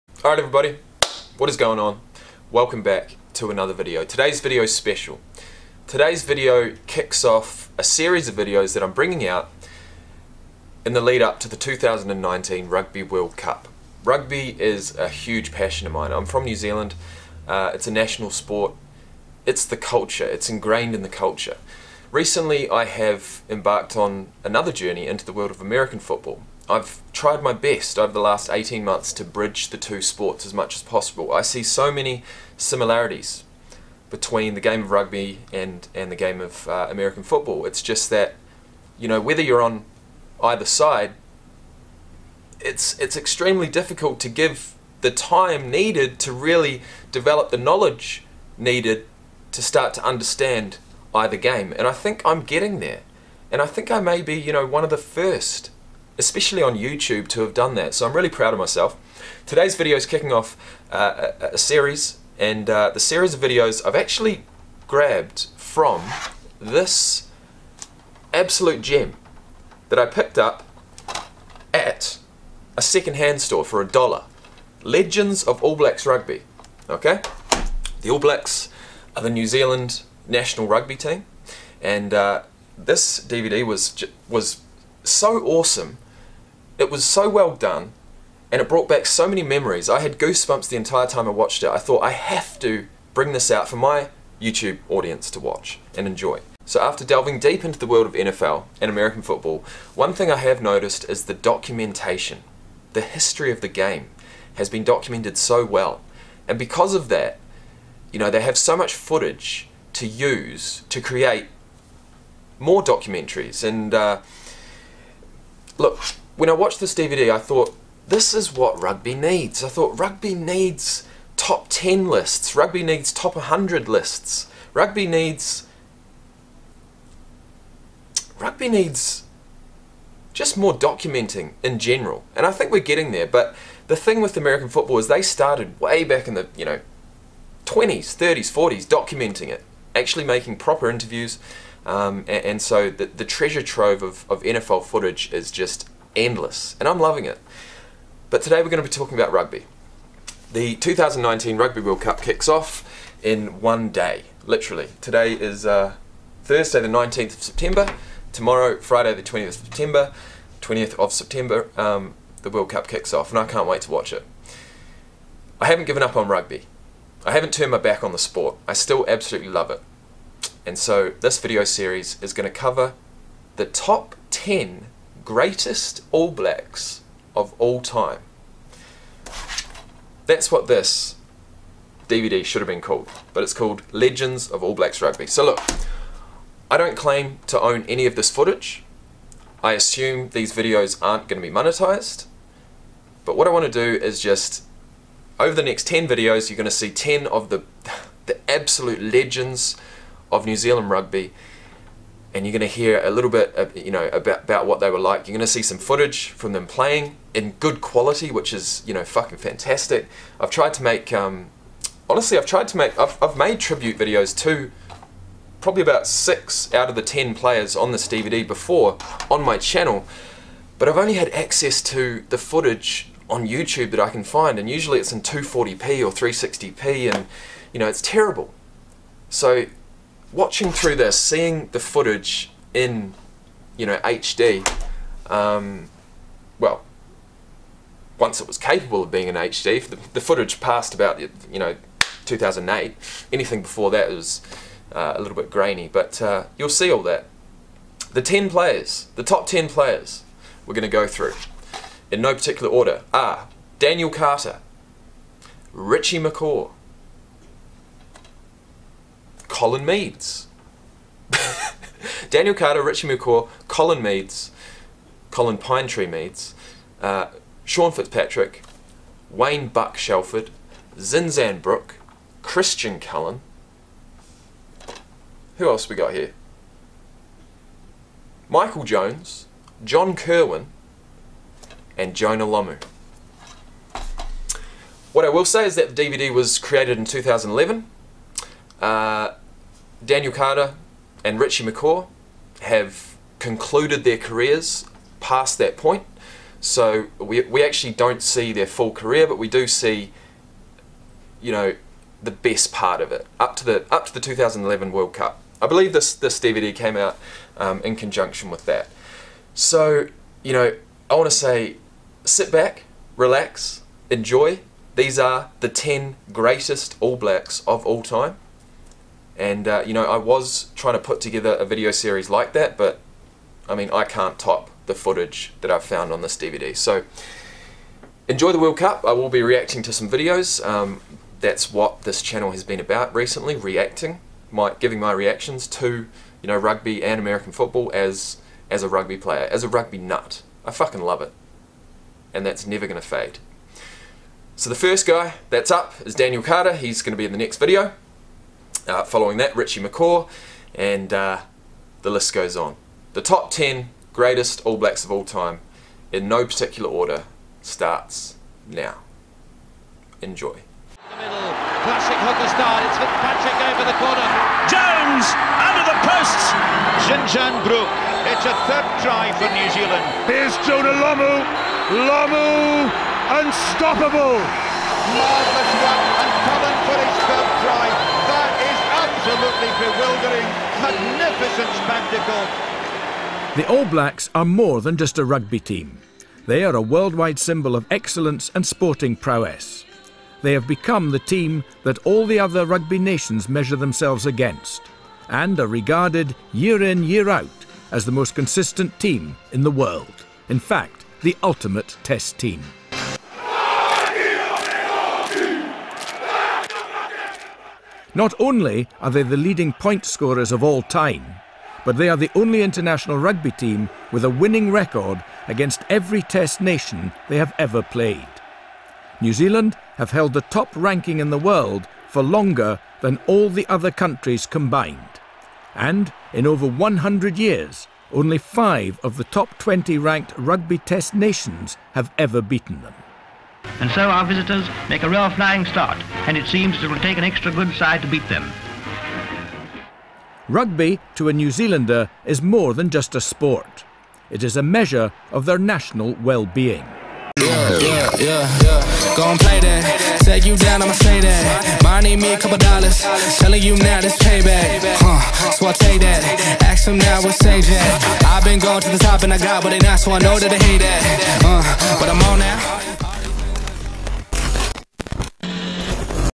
The Top 10 Greatest New Zealand All Blacks Of All Time Video Series!
This is a passion project of mine I've put together to coincide with the start of the 2019 Rugby World Cup which begins today!

GO ALL BLACKS!

The Top 10 Greatest New Zealand All Blacks Of All Time are... (In no particular order)

DANIEL CARTER
RICHIE MCCAW
WAYNE SHELFORD
JOHN KIRWAN
MICHAEL JONES
CHRISTIAN CULLEN
JONAH LOMU
COLIN MEADS
SEAN FITZPATRICK
ZINZAN BROOKE

Each and every one of them make me feel some kind of way, and that is what rugby is all about.